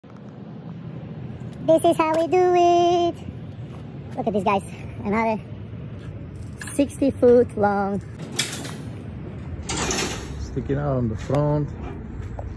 Trucking the flat bed !Long steel beams sticking out both sides
This 60ft long steel beams are getting popular !